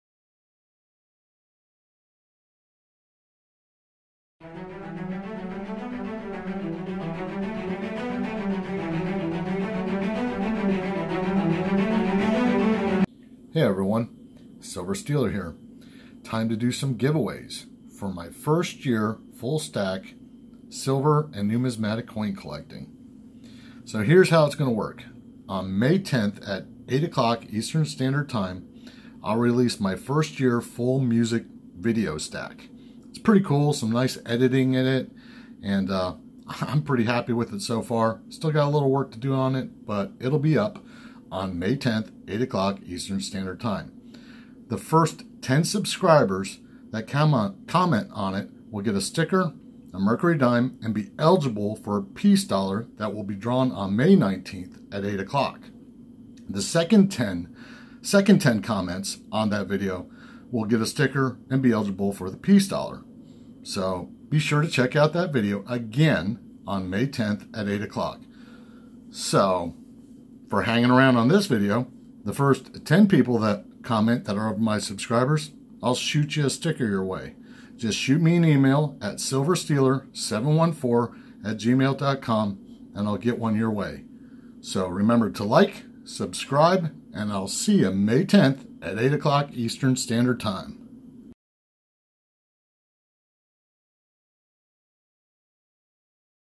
1st Year Stack Celebration GAW Announcement
Rules! You can comment on this vid once towards a sticker. Then comment once on May 10th towards the main prize. This will remain open until 20 spots are filled or until shooting of the drawing on May 19th 8pm est.

First 10 subscribers to comment on this video will receive one of my stickers.

On may 10th at 8pm est My full stack vid will air.

The first ten subscribers that comment will receive a sticker and a Mercury Dime and be entered into the drawing on May 19th 8pm est

The next 10 subscribers that comment will receive a sticker and be entered into the Peace Dollar Drawing held on May 19th 8pm est.

The Peace Dollar: 1923 Philly. A white Blazer and will at least get a ms 63 or better imo.

There will be 20 different Subscribers entered for the Peace dollar in total. Vid will be shot on May 19th 8pm est sharp. It will be timestamped to be fair and uploaded as quickly as I can. Winners shoot me an email to mail it to you! Addy at top.